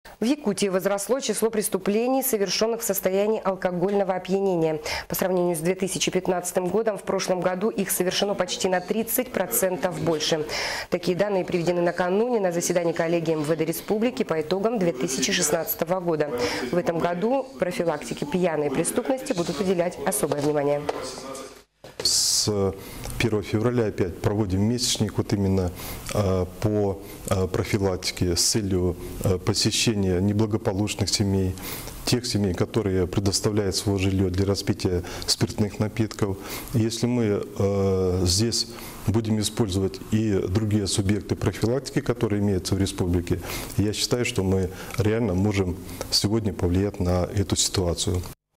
В Якутии "пьяной" преступности будут уделять особое внимание
Официальный сайт NVK-ONLINE.RU - В Якутии возросло число преступлений, совершенных в состоянии алкогольного опьянения. По сравнению с 2015 годом в прошлом году их совершено почти на 30% больше.
Такие данные приведены накануне на заседании коллегии МВД Якутии по итогам 2016 года.